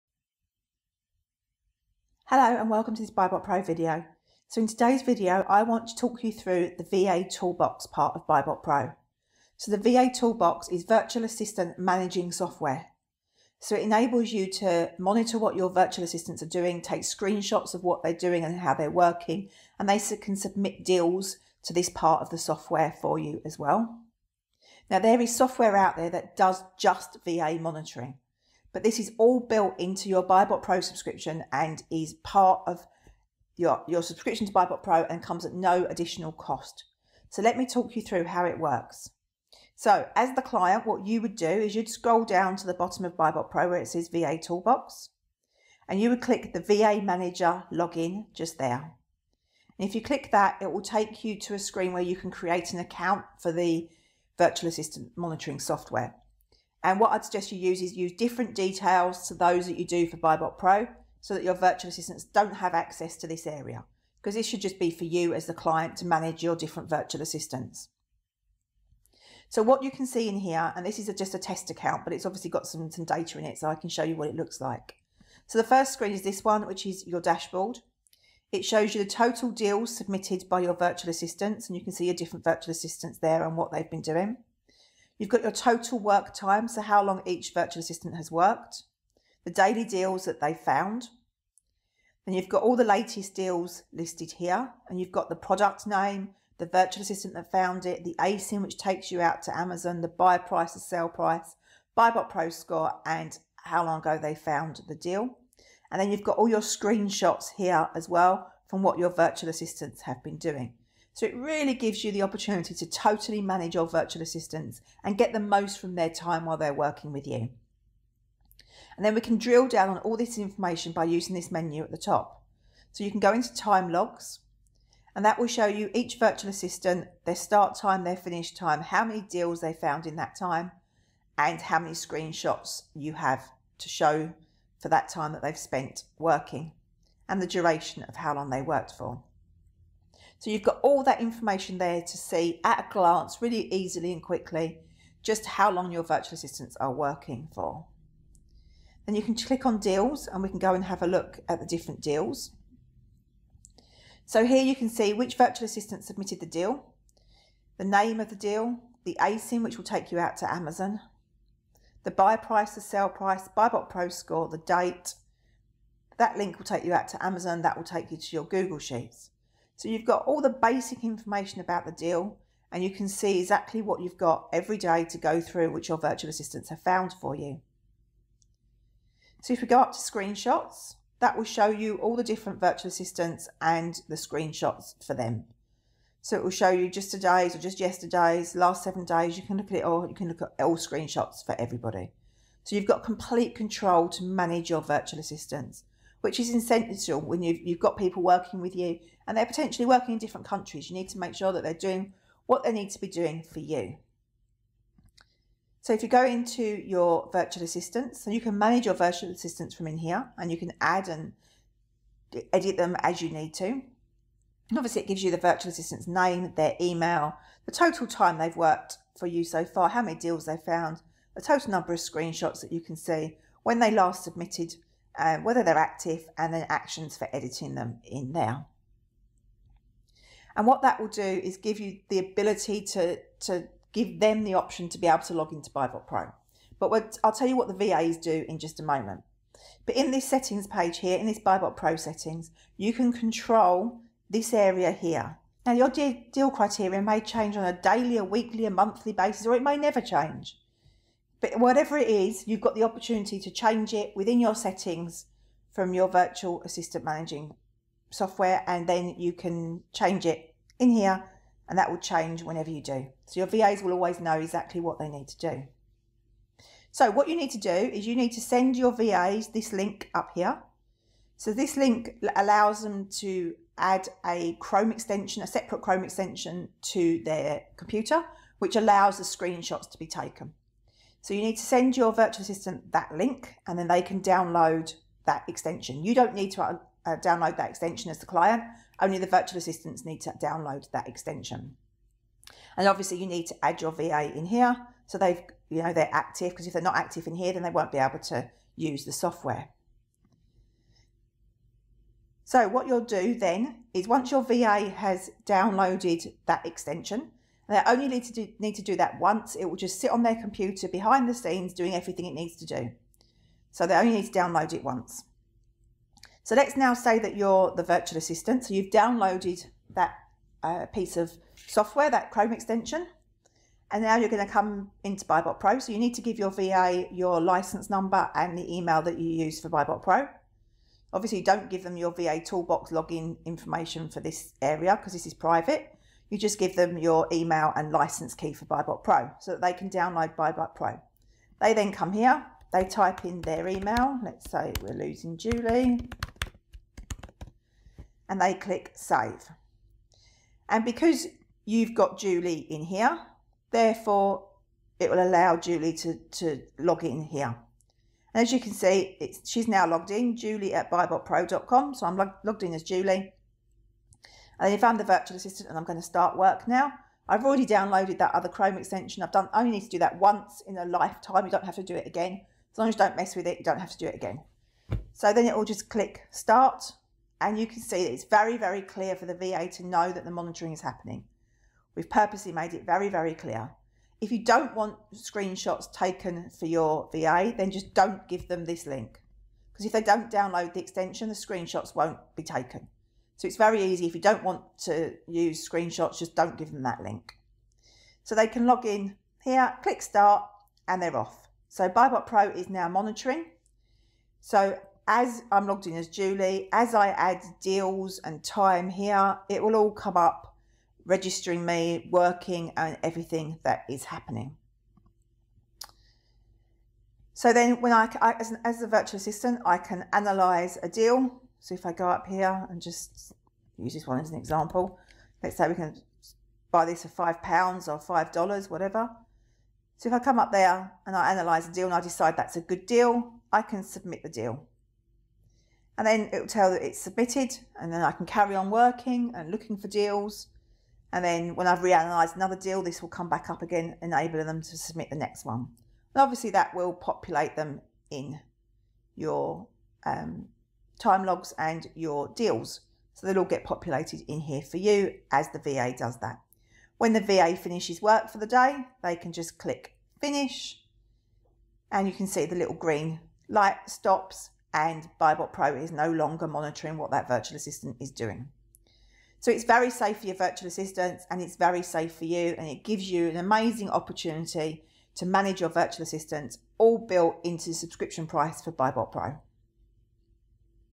VA Toolbox - BuyBotPro Mini Video Training Session
Want to manage your VAs with time logs and screenshots?!

BuyBotPro has it all built in!

Full VA Management tool with screenshots, time logs, date stamps, deal criteria list and deal database!

Never again wonder what your VA is doing with their time - see it for yourself!

All included within your BuyBotPro Subscription.